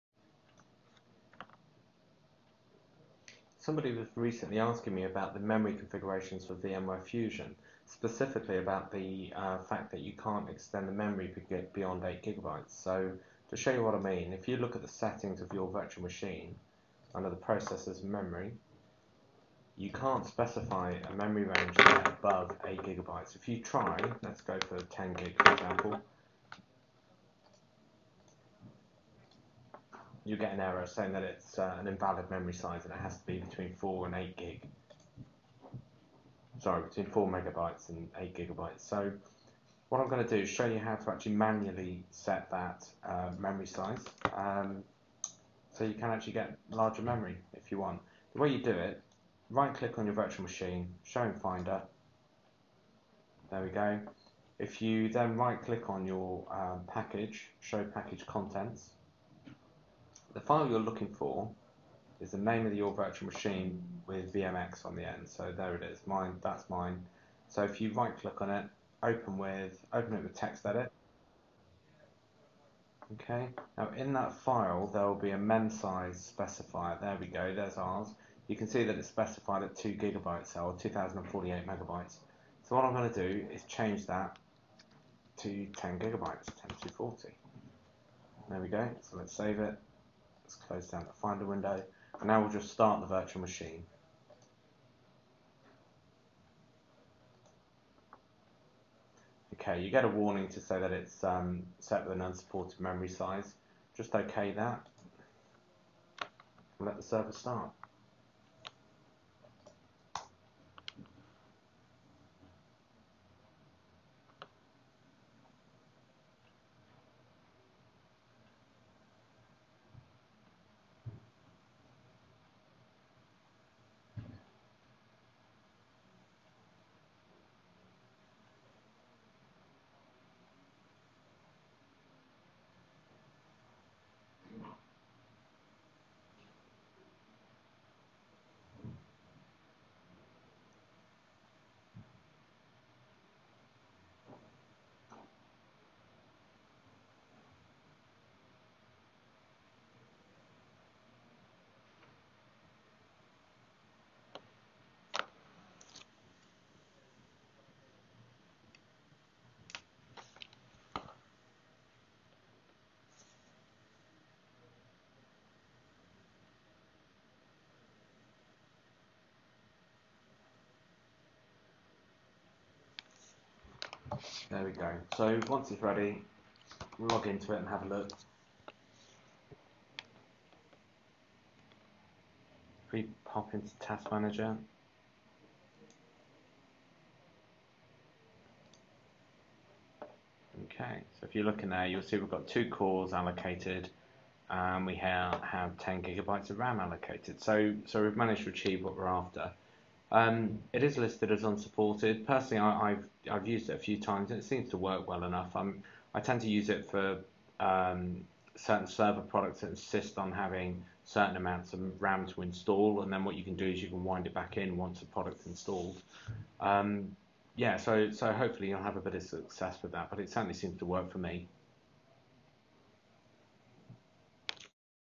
Large RAM Configuration (VMWare Fusion)
Run through of how to allocate more than 8Gb of RAM to a guest in VMWare Fusion.